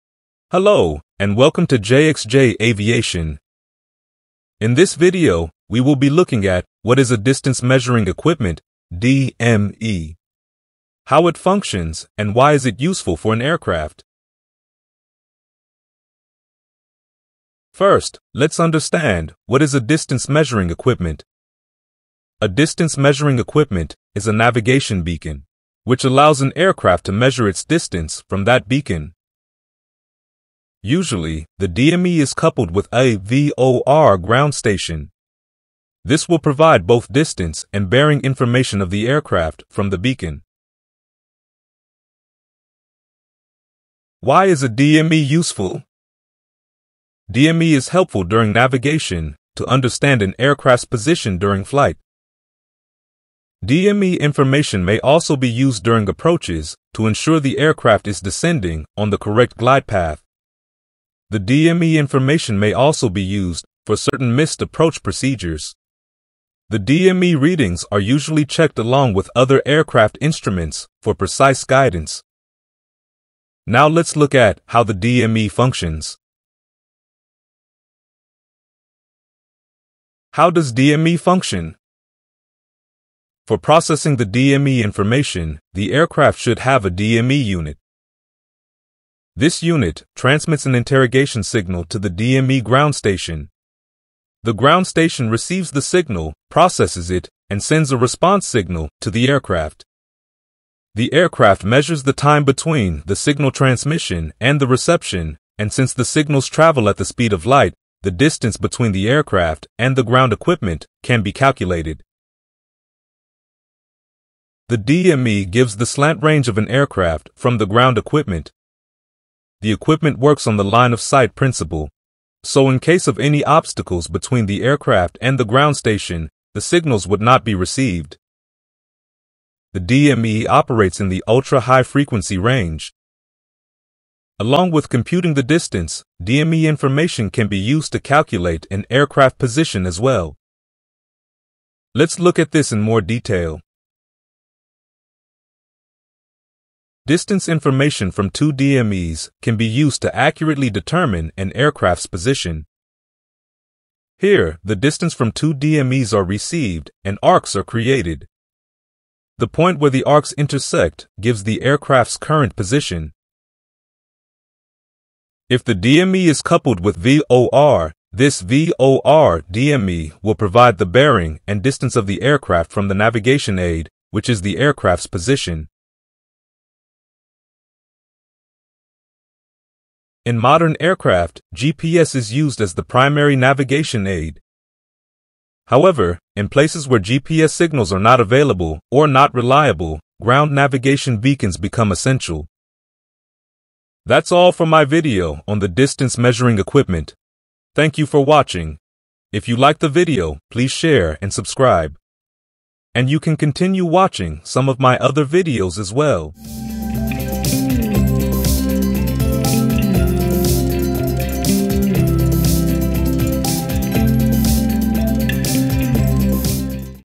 What is a Distance Measuring Equipment? | How DME is used to calculate Aircraft Position
Hello. In this video we look at what is a Distance Measuring Equipment or DME. We see how a DME is used by an Aircraft to compute its distance from this beacon. The distance information may be used for different purposes which is given in this video.
We also look at another important use of the DME, which is calculating an aircraft's position. This video just gives a 2d idea of position computation. However in a practical scenario, a 3d computation has to be done to calculate an aircraft's position.